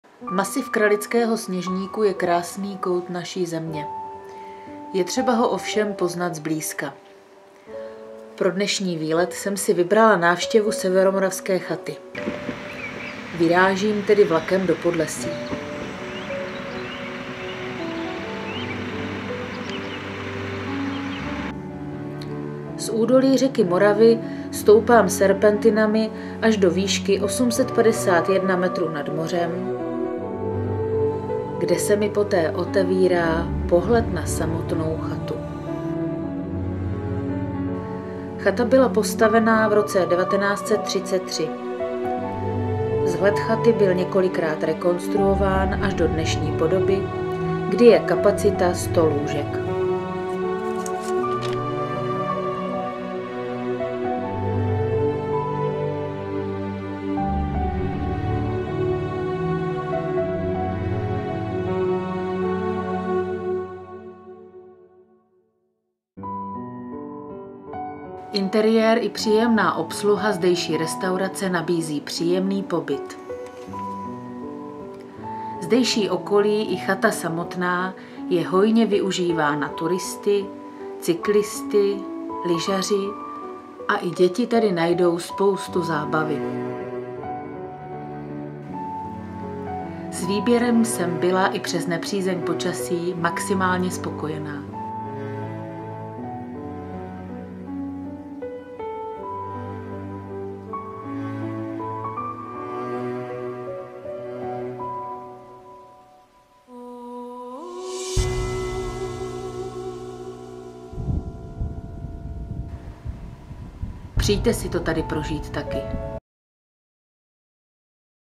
Národní pohádky a pověsti: Mediální workshop Severomoravská chata duben 2014
Více než dvě desítky zájemců se zúčastnilo počátkem dubna 2014 mediálního workshopu v rámci projektu Národní pohádky a pověsti - poklady národní identity a tradic na Severomoravské chatě v Malé Moravě. Lektor, zkušený dokumentarista Marek Dušák je učil pracovat s kamerou a následně stříhat natočený materiál do žánrových dokumentů.  Výsledkem byla i pozvánka na Severomoravskou chatu.